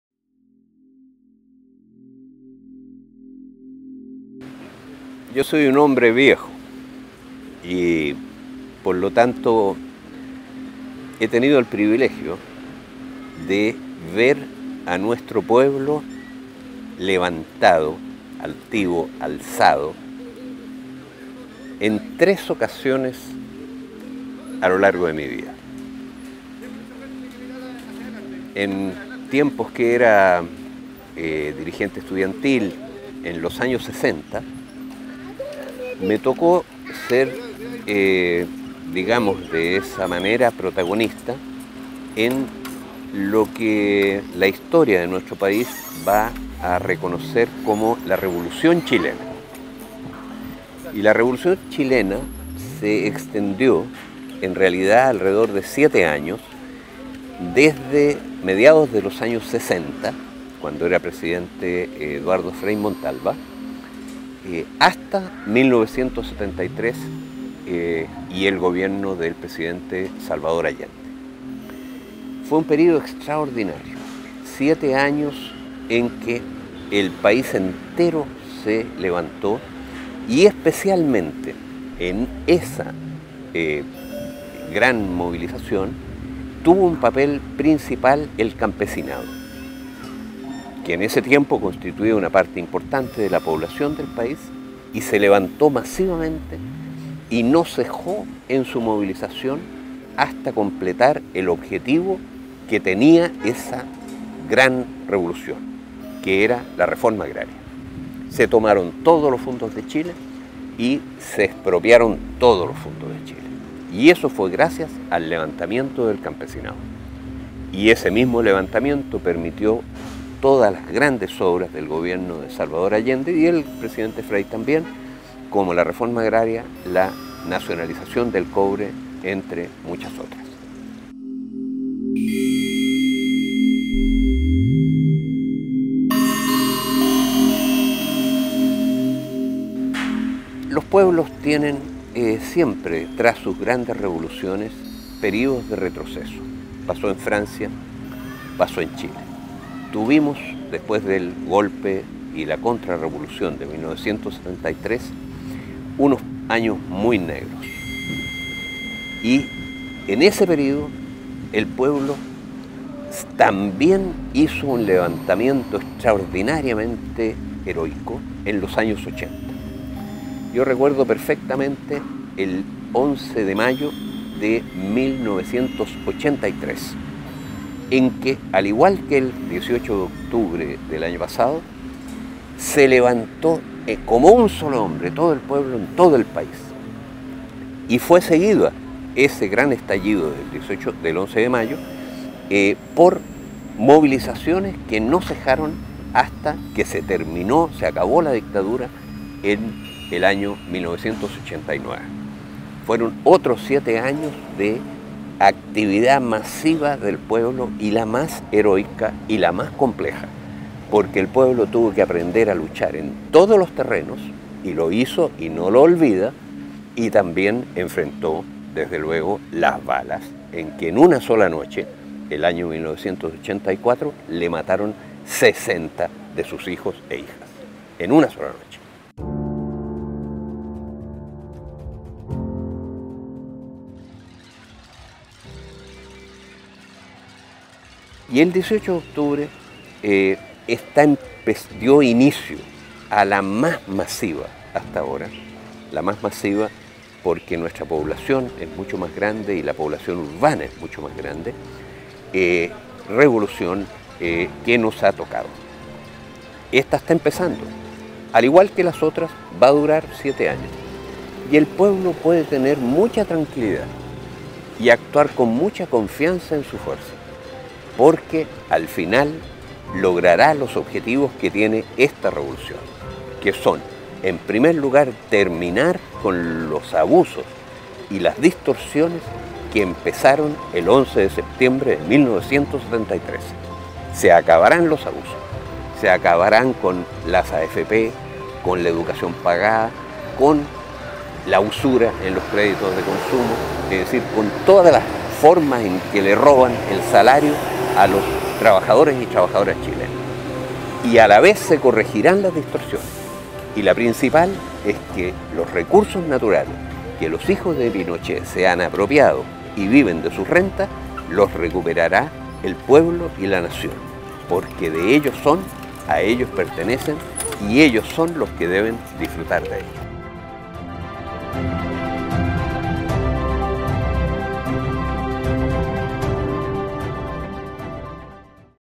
Economista afirma: "Esta revolución durará siete años"
Manuel Riesco ha sido testigo y actor de parte importante de las transformaciones políticas, económicas, sociales y culturales que explican el panorama contingente que vive Chile. "Soy un hombre viejo. Por lo tanto, he tenido el privilegio de ver al pueblo levantado, altivo, alzado en tres ocasiones a lo largo de mi vida". En este reportaje, el reconocido economista repasa esos tres grandes hitos de la historia nacional, realizando un pronóstico contundente: "esta revolución, al igual que las otras, durará siete años".